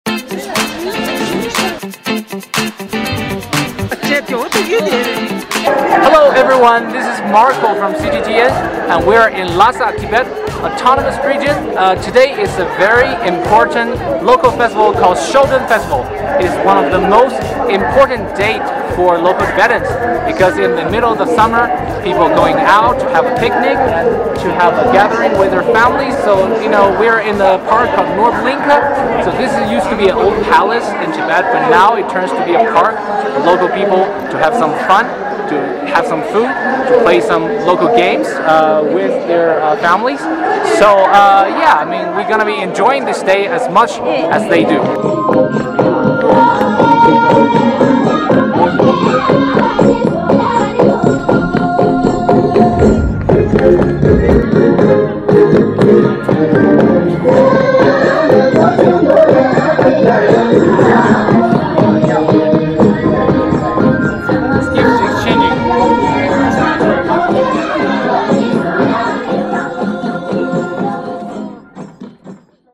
Picnicking in Lhasa: Celebrating Shoton Festival in Tibetan style
Marco and Oscar, aka “the beard brothers,” join local Tibetans and internet influencers for an authentic “Lingka” experience during the Shoton Festival — dancing, picnicking, chess playing and more…